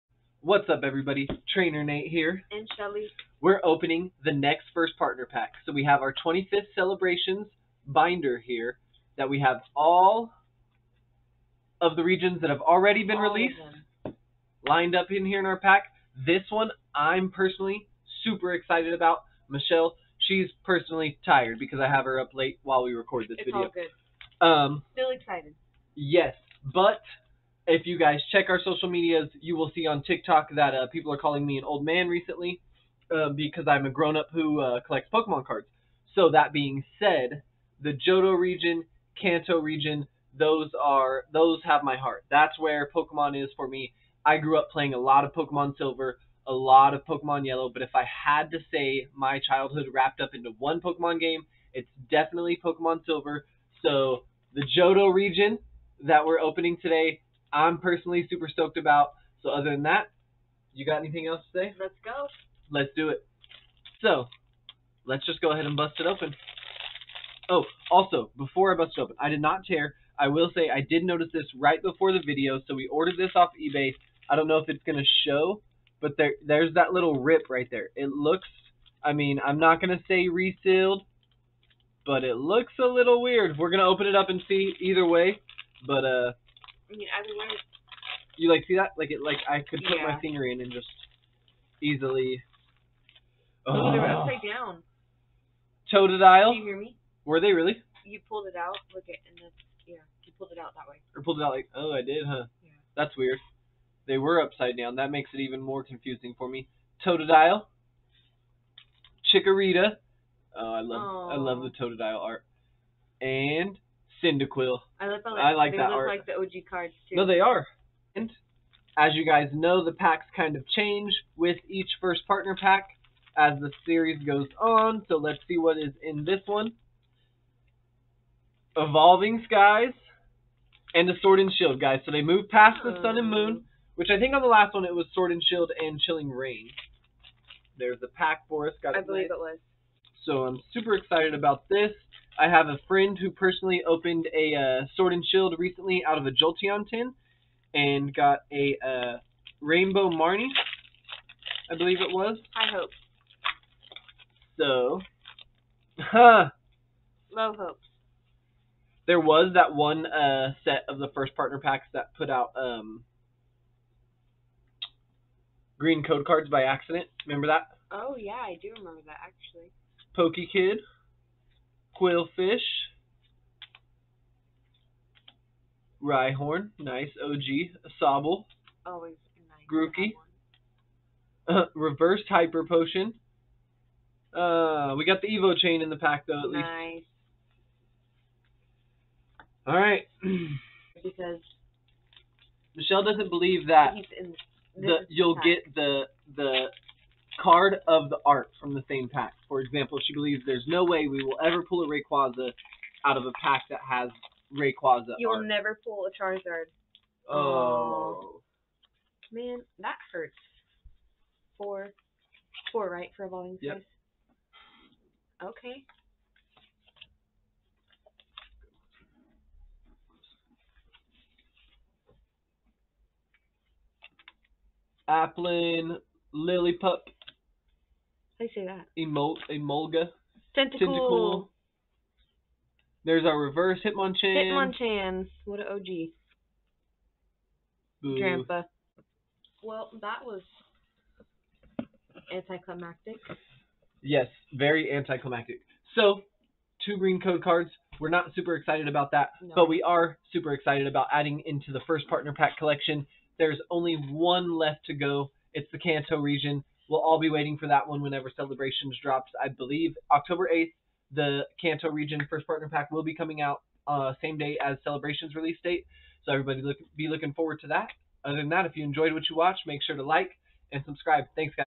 Let's open a Johto region first partner pack!!
Take a trip with us to the Johto region on this one while we open another first partner pack. All of the clips come directly from our twitch channel live come have some fun with us!